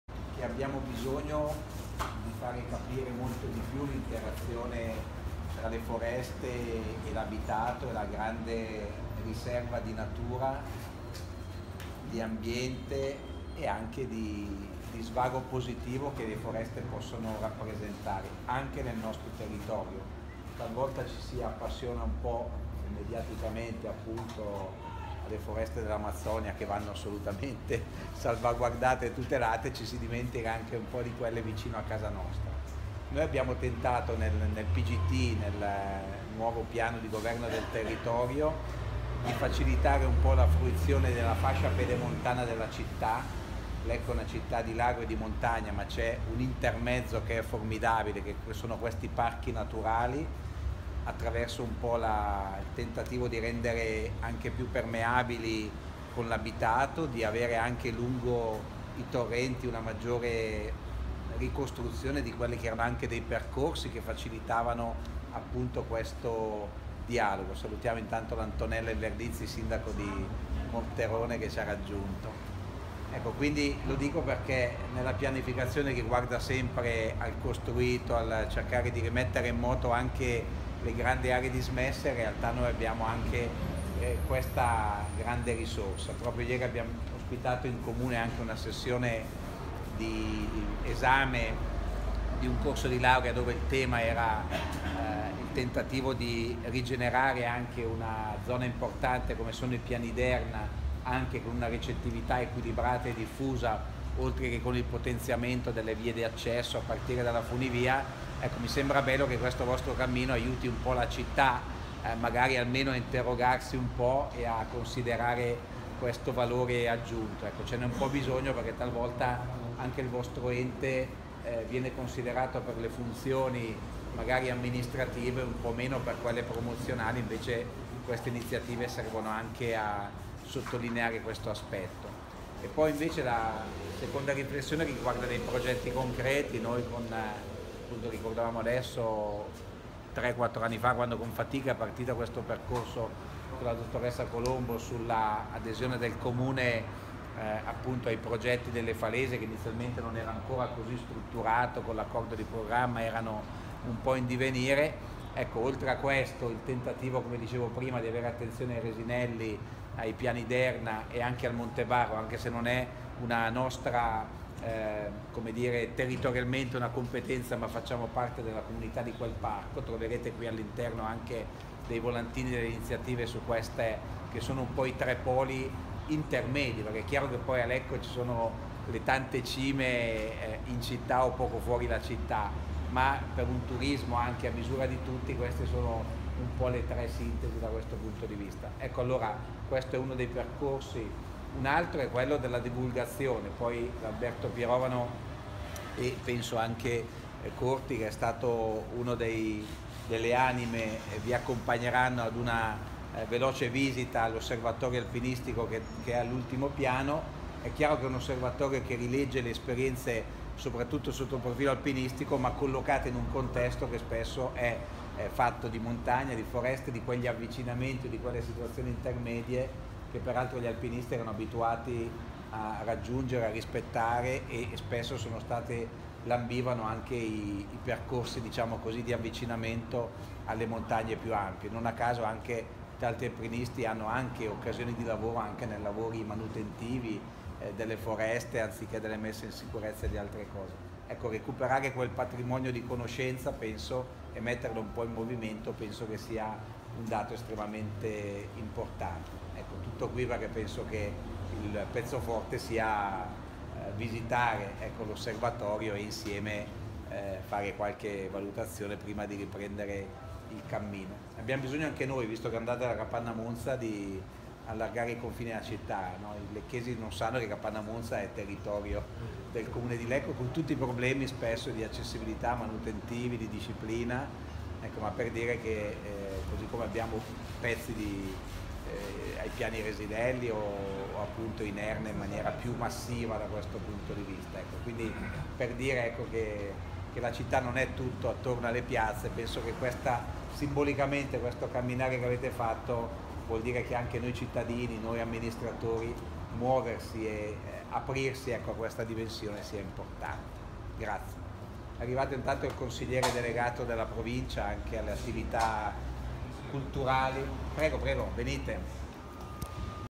Il sindaco di Lecco Virginio Brivio
Il sindaco do Lecco Virginio Brivio, incontra Cammina Foreste Lombardia e Ersaf